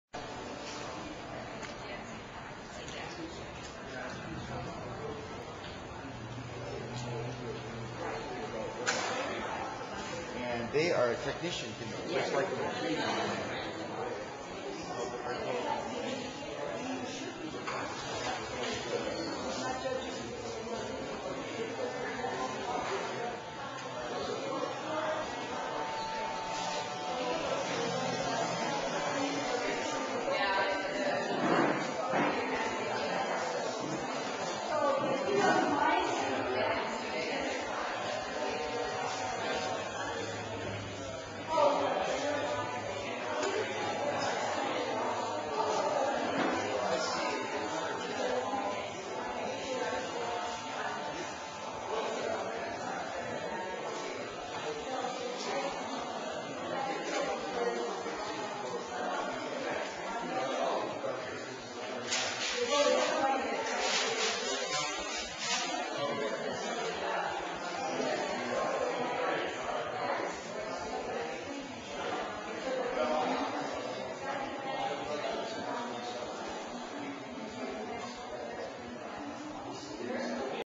Take-in: 32nd Annual Fall Arts Festival, Hilo, Hawaii 's East Hawaii Cultural Center 2008
Over 200 pieces of art were entered into this year's juried exhibition for Hawaii Island artists in all media.  81 pieces were chosen for the 32nd Annual Big Island Fall Arts Festival, which opens November 7, 2008 at 5:30pm.  Three award s are given by this year's Juror Kolbe.